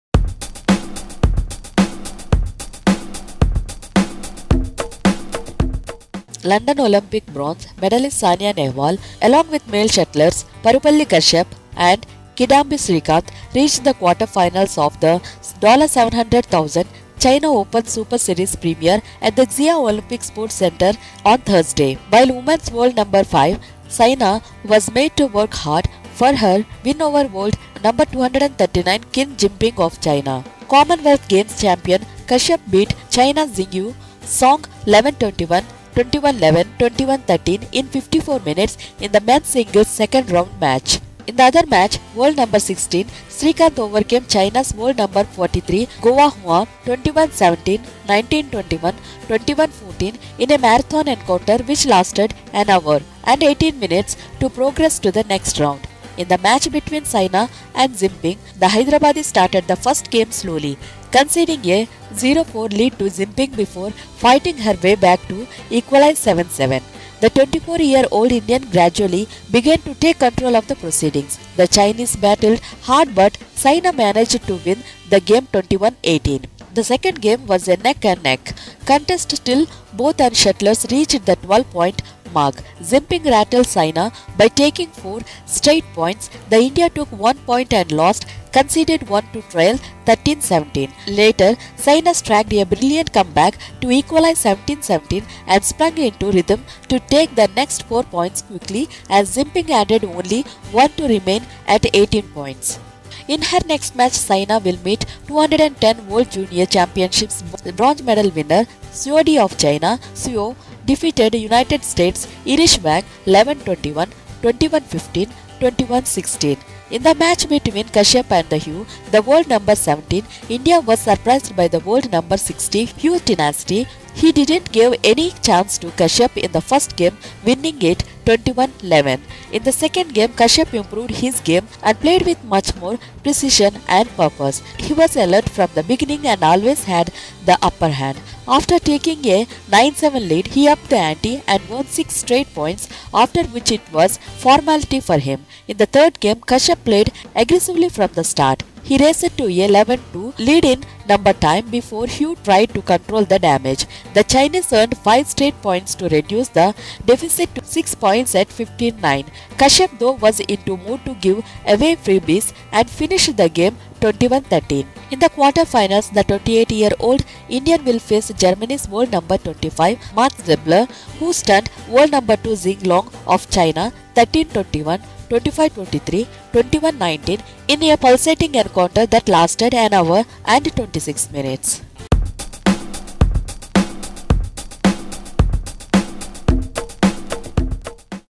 Saina, Kashyap, Srikanth in Quarters of China Open
London Olympic bronze medallist Saina Nehwal along with male shuttlers Parupalli Kashyap and Kidambi Srikanth, reached the quarter-finals of the $700,000 China Open Super Series Premier at the Haixia Olympic Sport Center here Thursday. While women's World No.5 Saina was made to work hard for her 21-18, 21-18 win over World No.239 Qin Jinping of China, Commonwealth Games champion Kashyap beat China's Xue Song 11-21, 21-11, 21-13 in 54 minutes in the men's singles second round match. In the other match, World No.16 Srikanth overcame China's World No.43 Gao Huan 21-17, 19-21, 21-14 in a marathon encounter which lasted an hour and 18 minutes to progress to the next round. In the match between Saina and Jinping, the Hyderabadi started the first game slowly, conceding a 0-4 lead to Jinping before fighting her way back to equalise 7-7. The 24-year-old Indian gradually began to take control of the proceedings. The Chinese battled hard but Saina managed to win the game 21-18.